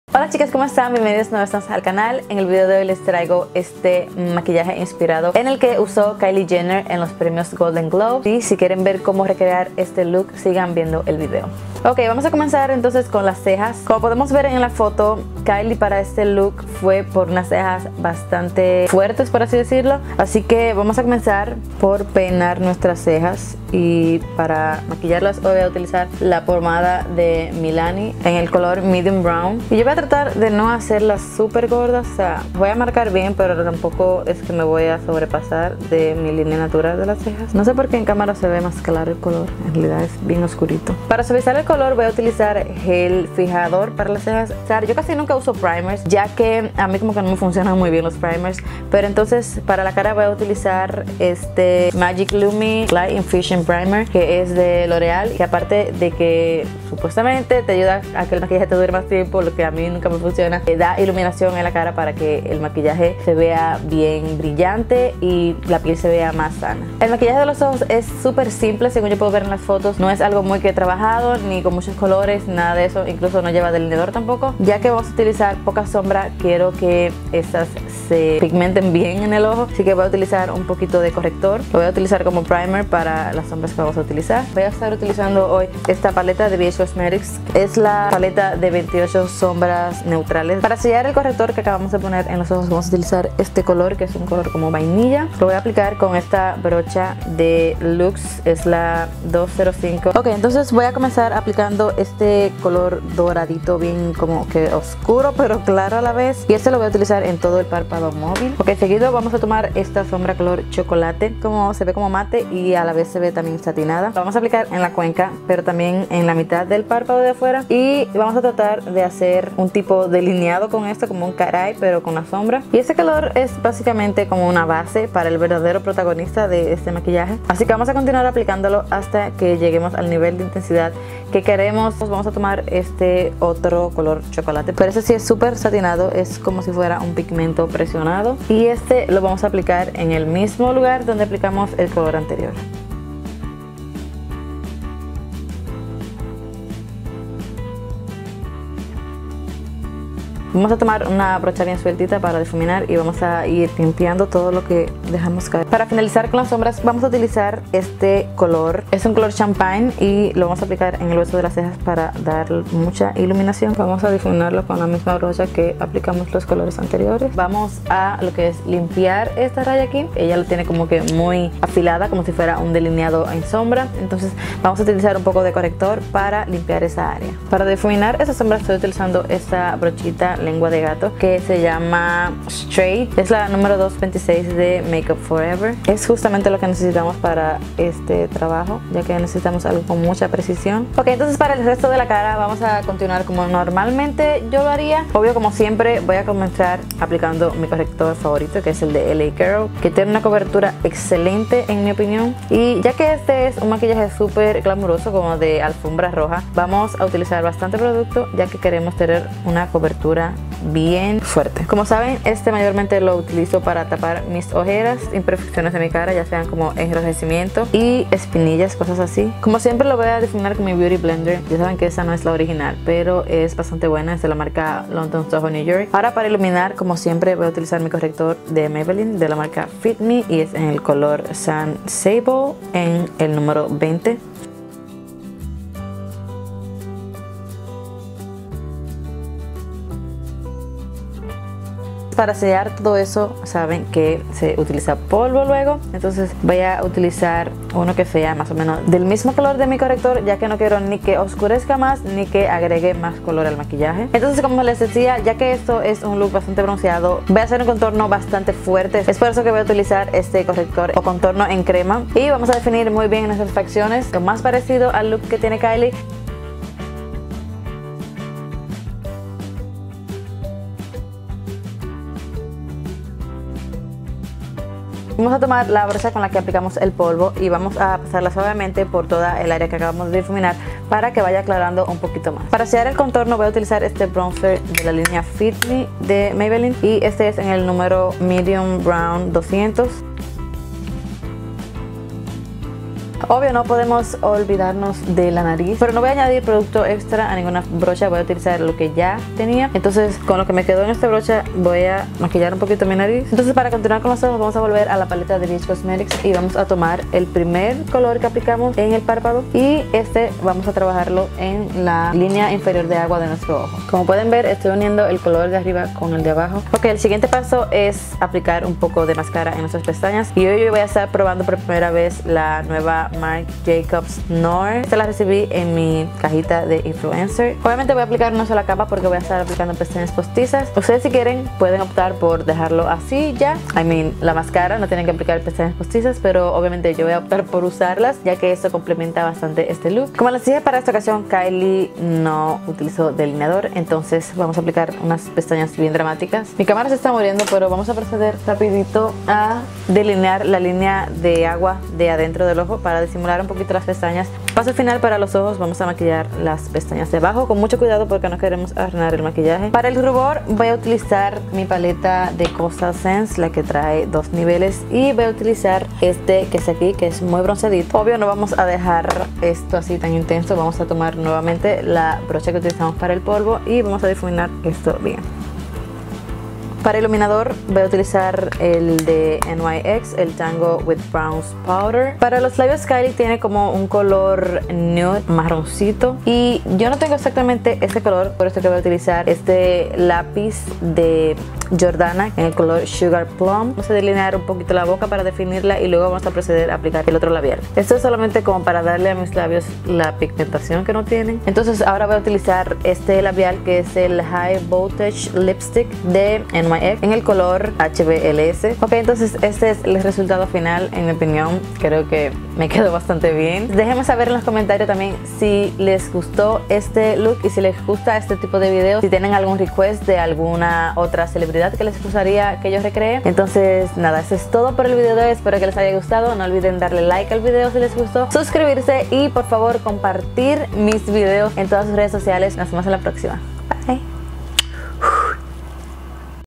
KYLIE JENNER GOLDEN GLOBES INSPIRED MAKEUP | MAQUILLAJE INSPIRADO EN KYLIE JENNER | KATH SANTANA
Hola hermosas!😊 Me encanto este look que uso Kylie Jenner en los premios Golden Globes asi que decidi recrearlo, es un maquillaje super facil de repetir. Espero que les guste mucho!😁💋

❤️SnapChat kathalay

Mayoría De Mis Maquillajes Y Brochas Son De

Me Gusta La Ropa De